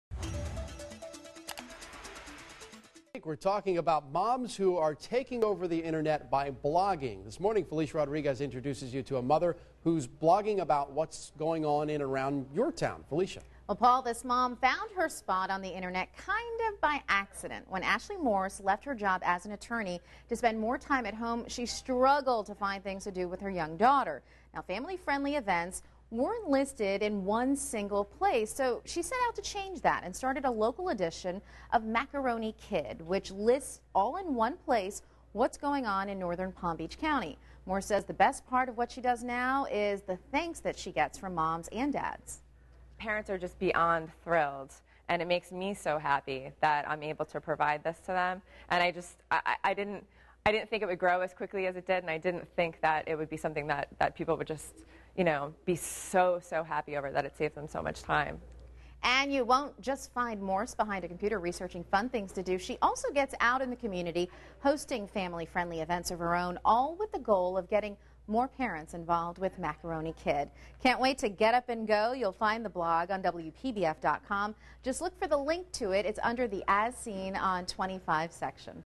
Moms Who Blog: Macaroni Kid
WPBF 25 News Mornings is introducing you to local "Moms Who Blog." Ashley Morse writes for